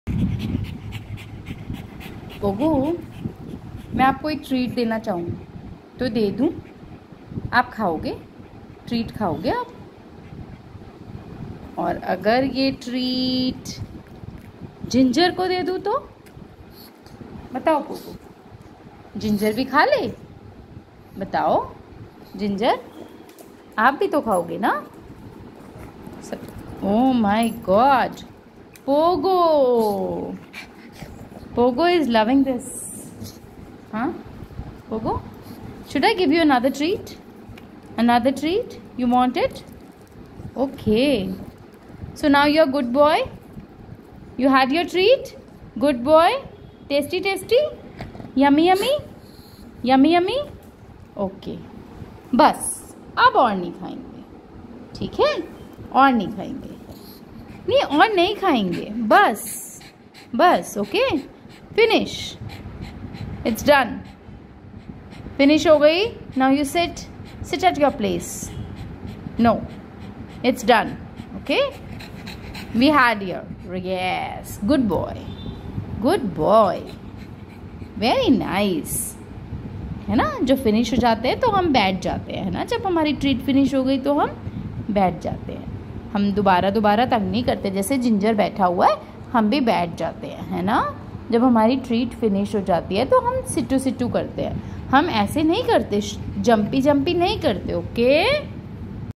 8 October 2024
Obedient pogo having treat. Treat ke liye upun kuch bhi karega